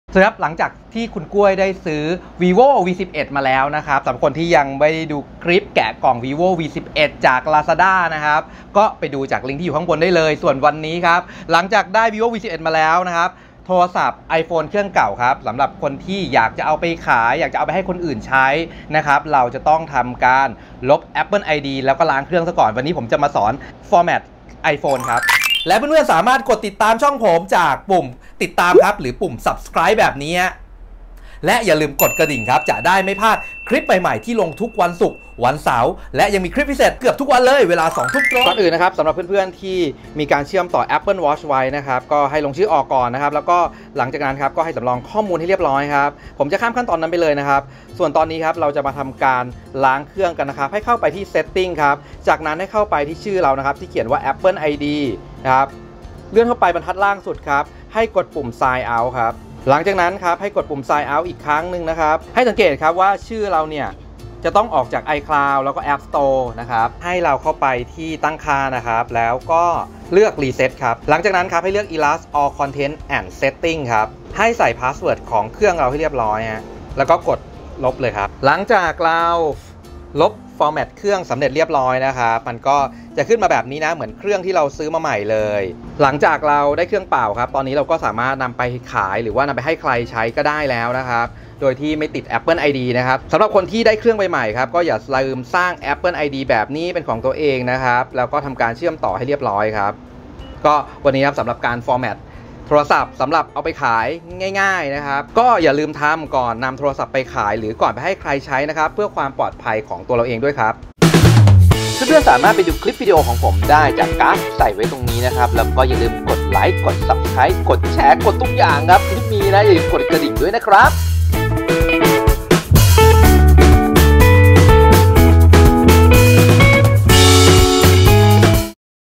วิธีล้างเครื่อง iPhone ก่อนเอาไปขายby T3B
ก่อนนำเครื่องไปขายหรอให้คนอื่นใช้อย่าลืม ลบข้อมูลออกจากเครื่องก่อนนะครับ